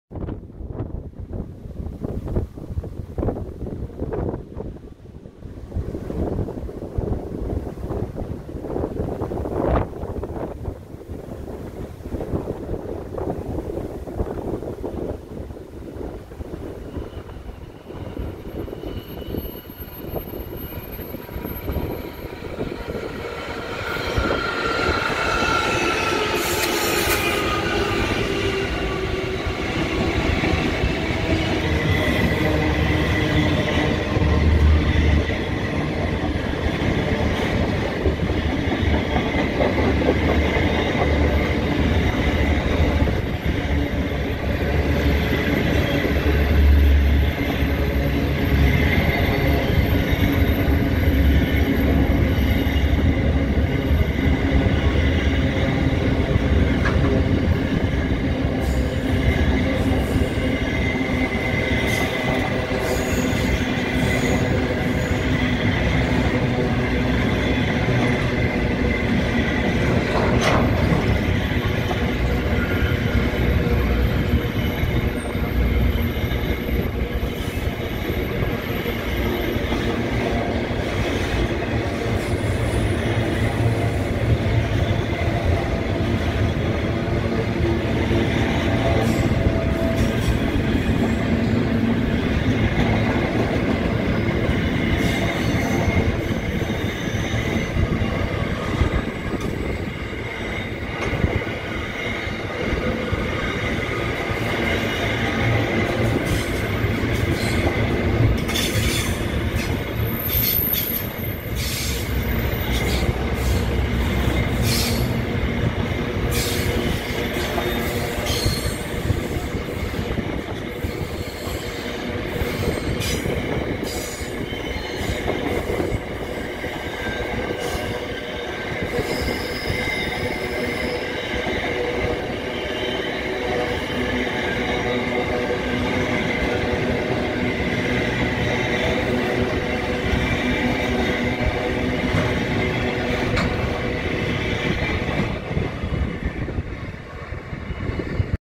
Pacific National Freight Train. Newcastle, Australia
Pacific National Freight Train pulled by four Diesel Locomotives travelling through the Hunter Valley town of Maitland NSW Australia 14/11/2021